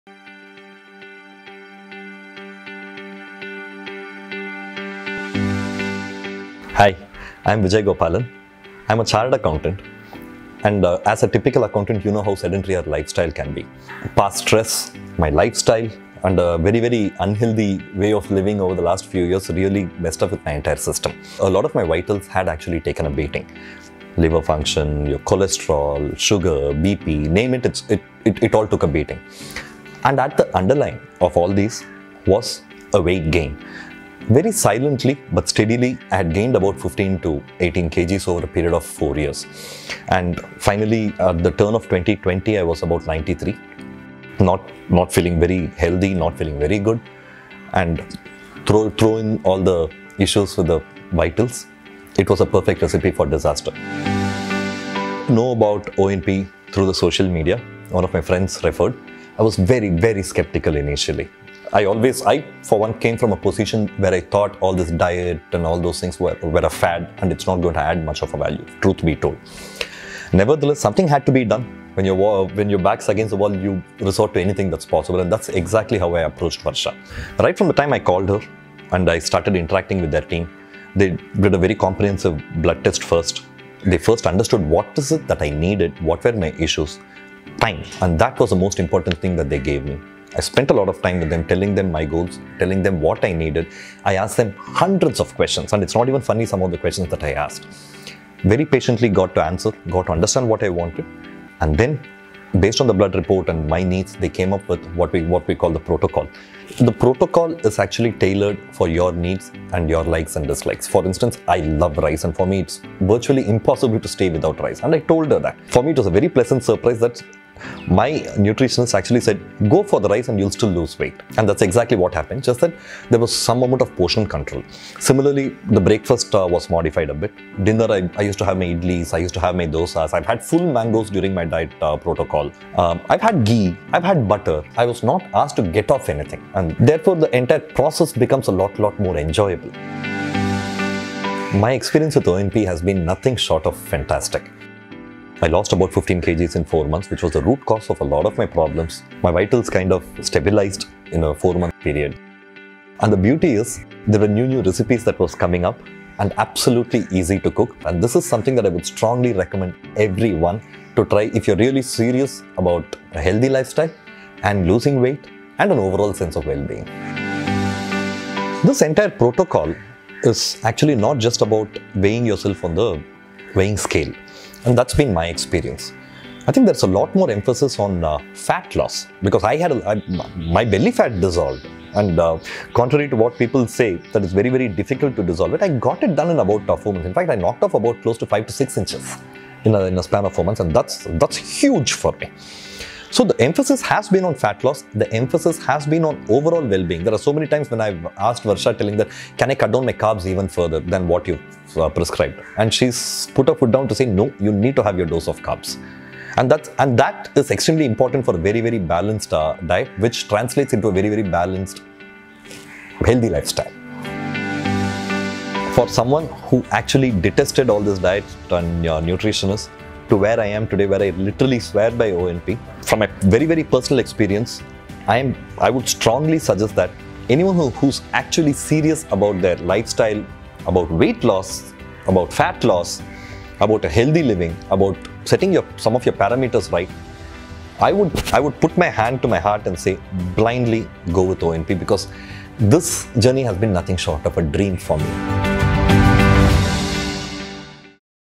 Science based body recomposition | Vijay Gopalan and his journey with Optimal Nutrition Protocol
True health and science based body recomposition, Vijay Gopalan and his journey with Optimal Nutrition Protocol